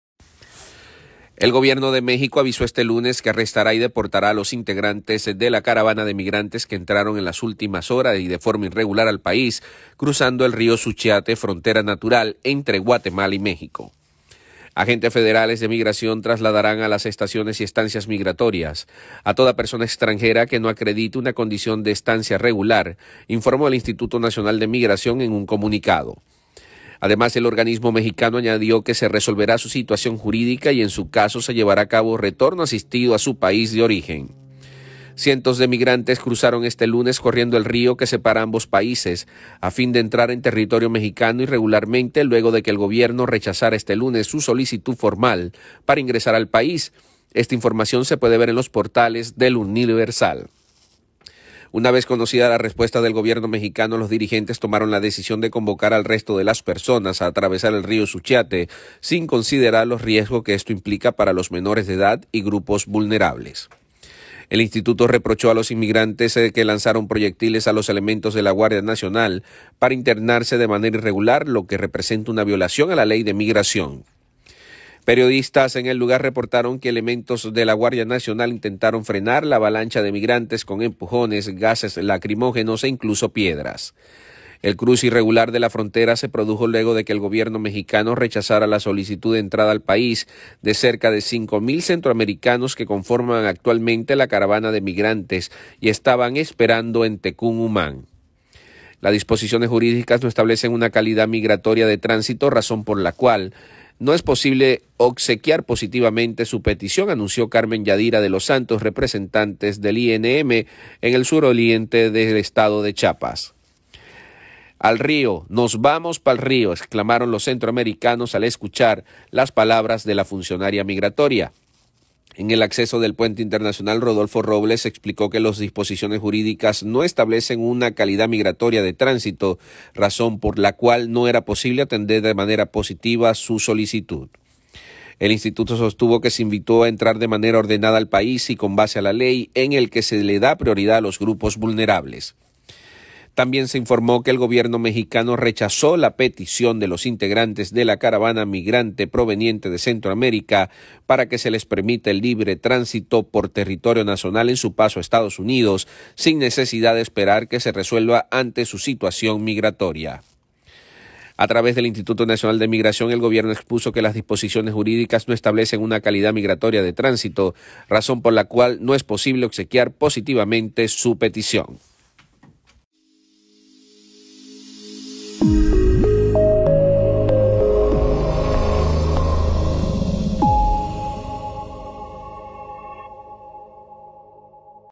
México arrestará y deportará a cientos de migrantes que entraron al país
México caza migrantes. El Gobierno de México avisó este lunes que arrestará y deportará a los integrantes de la caravana de migrantes que entraron en las últimas horas y forma irregular el país cruzando el río Suchiate, frontera natural entre Guatemala y México.

Cientos de migrantes cruzaron este lunes corriendo el río que separa ambos países a fin de entrar en territorio mexicano irregularmente luego de que el Gobierno rechazara este lunes su solicitud formal para ingresar al país. Esta información se puede ver en los portales de El Universal.